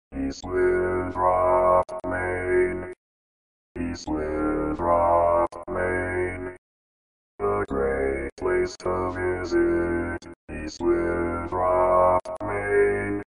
How to Say or Pronounce USA Cities — East Winthrop, Maine
This video shows you how to say or pronounce East Winthrop, Maine.

A computer said East Winthrop, Maine. How would you say East Winthrop, Maine?